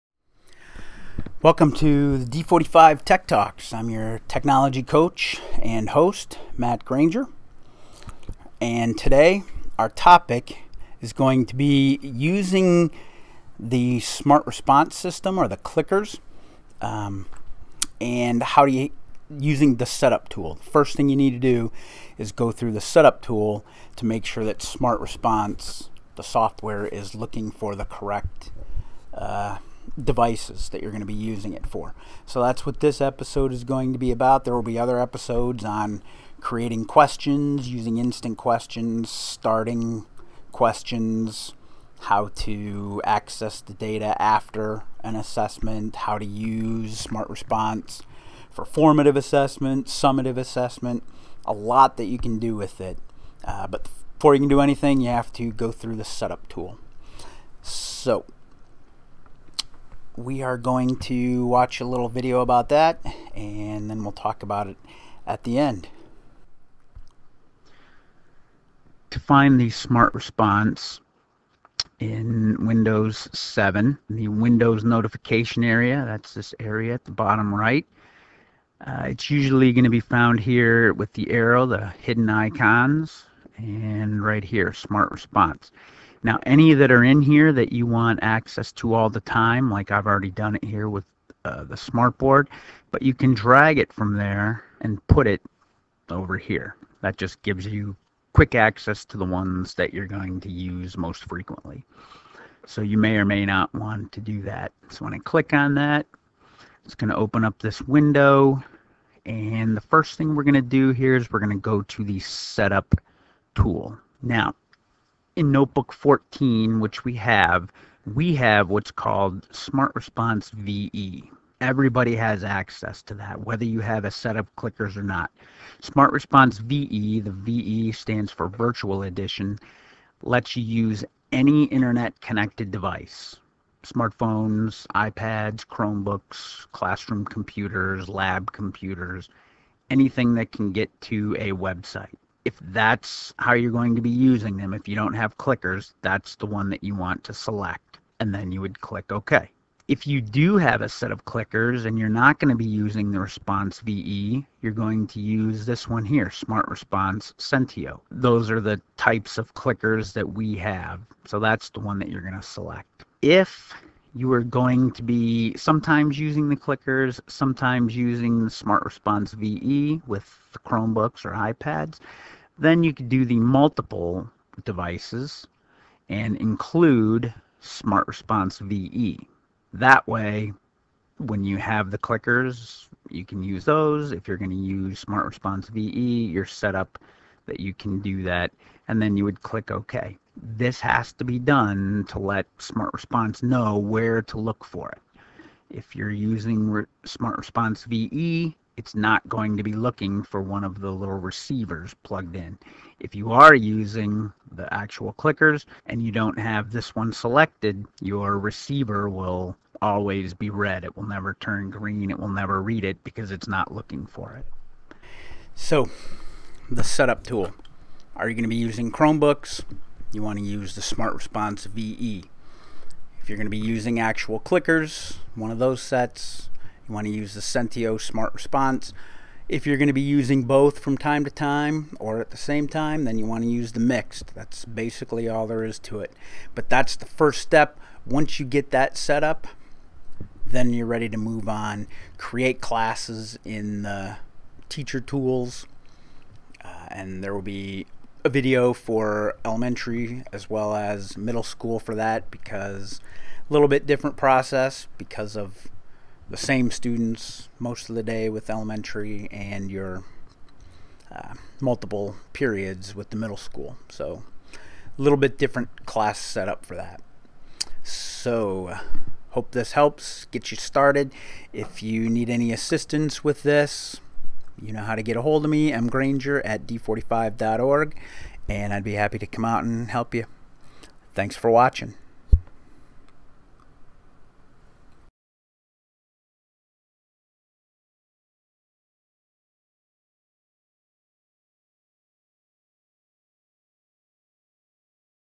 Using the SMART Board in Instruction: The SMART Response Set Up Tool
Before using SMART Response for assessments, you must use the set up tool to tell the software which model of SMART Response you will be using (actual clickers or the online VE method). If you don't do this first, you'll be sorry.